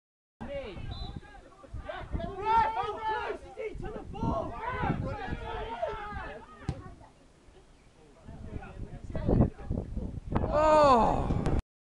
ECPL DIV1 GAME, St Stephen 1 v 2 St Minver
A Courtney Rowe free-kick is headed goalward's by Craig Coad but the St Minver keeper again saves the day.